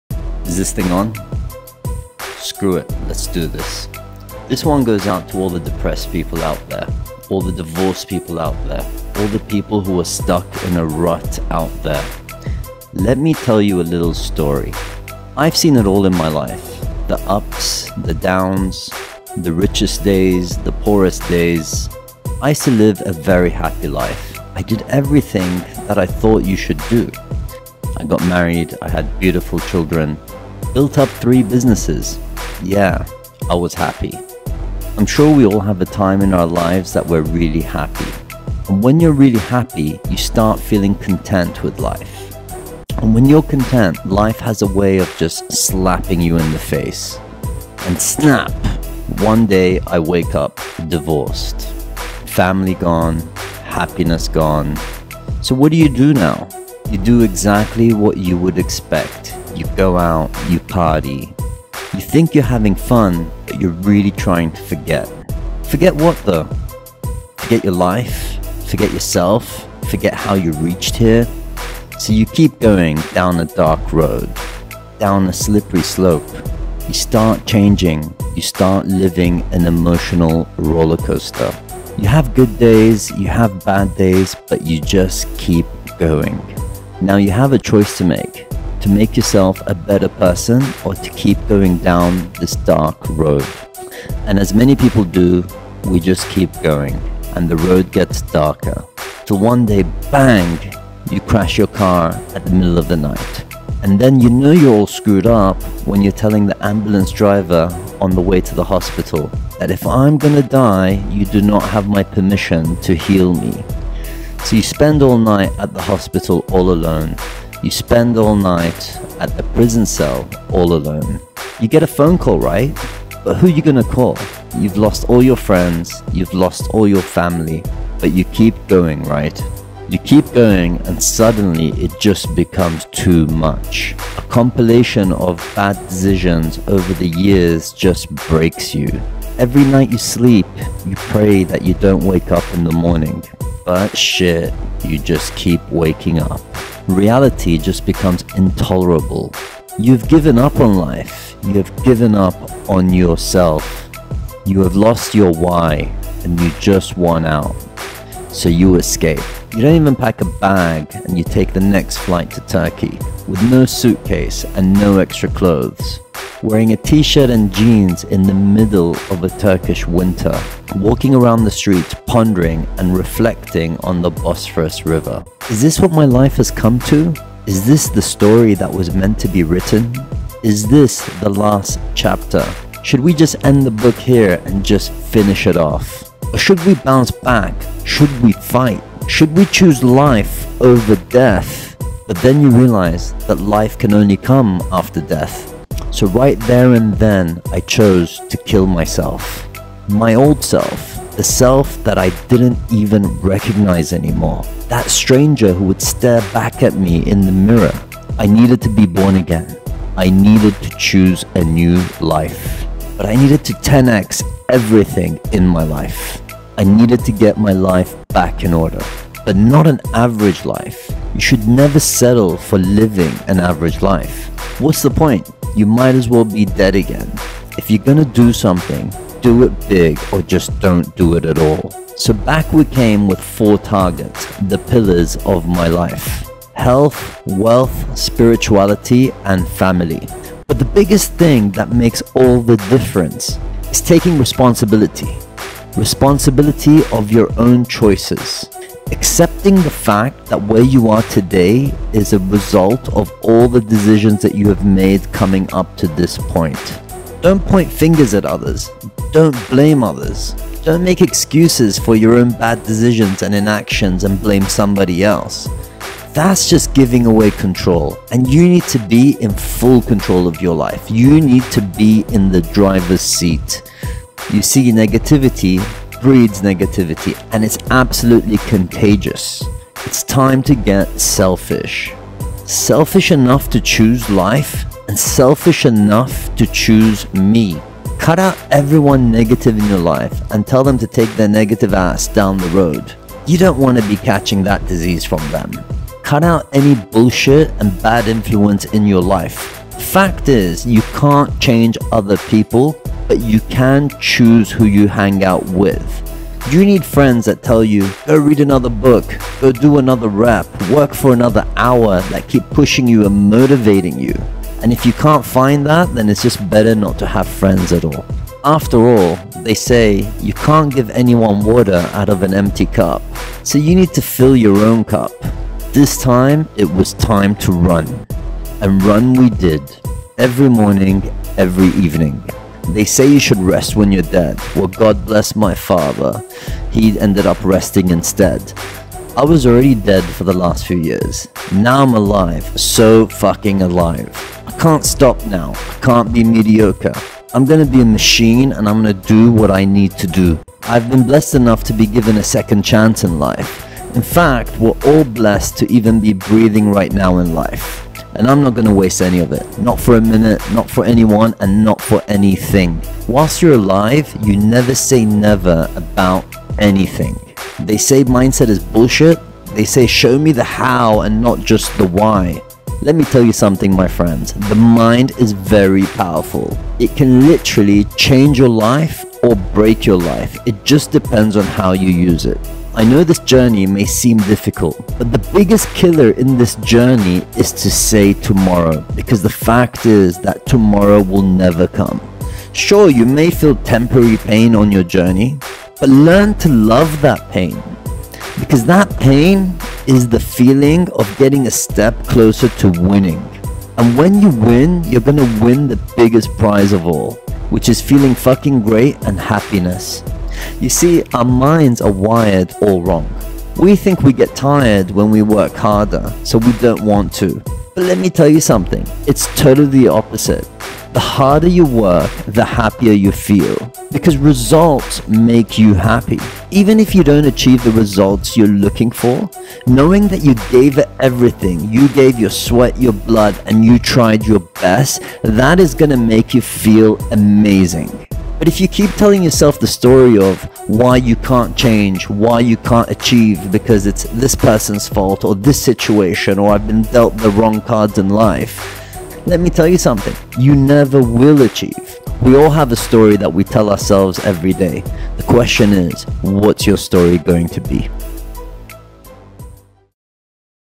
Don't Quit On Yourself - How To Beat Life When You're Down
It took a lot of courage to actually share this with you.

But sometimes, you need to be real and cut the crap from your social.

Life isn’t always perfect, but we can strive to make it that way.

If you are feeling depressed, have had a recent divorce or are just going through a tough time right now, remember, we all have had journeys, and sometimes sharing them can help others.

If there are any people out there, watching this, that may be hurting right now, I hope sharing this with you, will give you some inspiration.

If you think listening to this will help anyone you know, please share it with them.

Don’t go down the dark road and make that active choice today, to be the best version
of yourself.

Because as soon as you decide you’re ready and you find your “Why” again, I’m here to tell you that the “How” will happen naturally.

Don’t ever quit on yourself, love yourself and find yourself.

Much Love

Hussain Husain